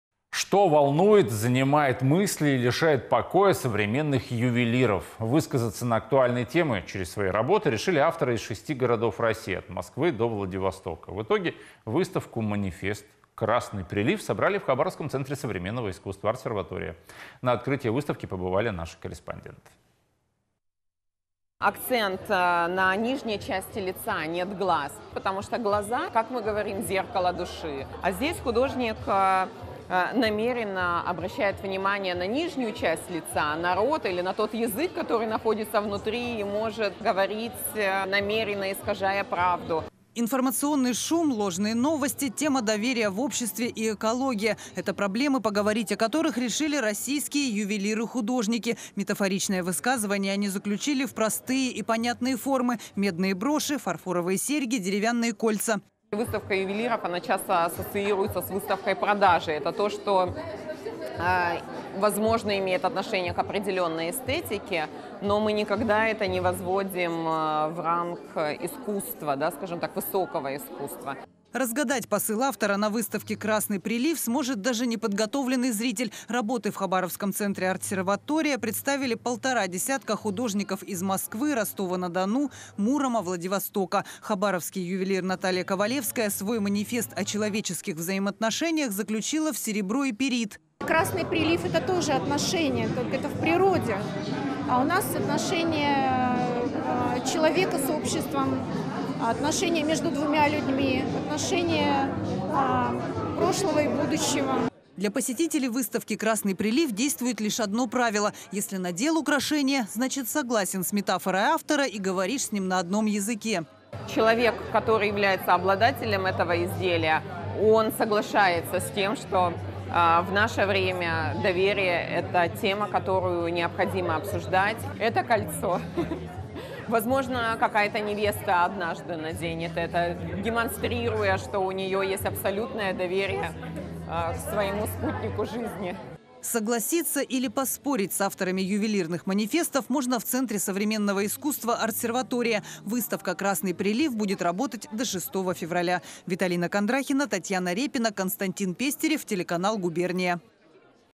Красный прилив: в Хабаровске прошла необычная выставка современных украшений. Новости.26/01/22
Что волнует, занимает мысли и лишает покоя современных ювелиров? Высказаться на актуальные темы через свои работы решили авторы из шести городов России: от Москвы до Владивостока. В итоге выставку-манифест «Красный прилив» собрали в хабаровском центре современного искусства «Артсерватория».